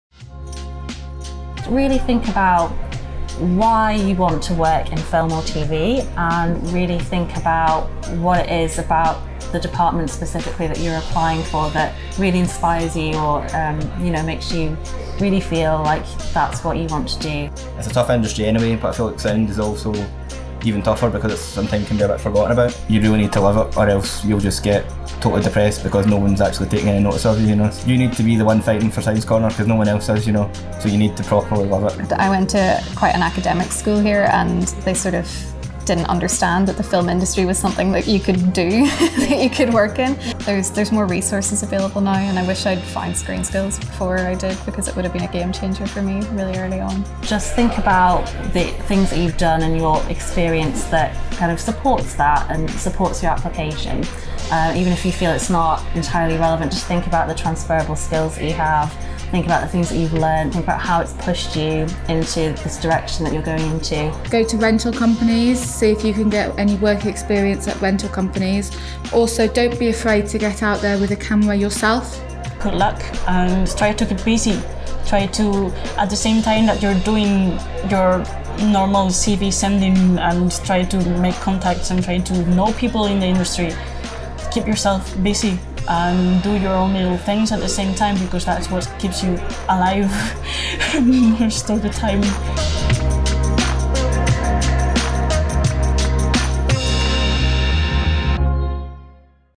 How to get your first job in film
Five film and TV professionals give their tips on how to get your first job in film. They talk about knowing which department you want to work in and loving it, using the resources from ScreenSkills to help yourself, how to get work experience, and how it is good to make your own work.

Interviewees:

Mary-Jean Walsh - costume assistant

Aiden McDonach - sound assistant

Julie McCausland - costume assistant

Charlotte Austwick - camera assistant

Iguácel Cuiral - camera assistant

Look at our upcoming events where you can connect with, and get advice from, industry professionals: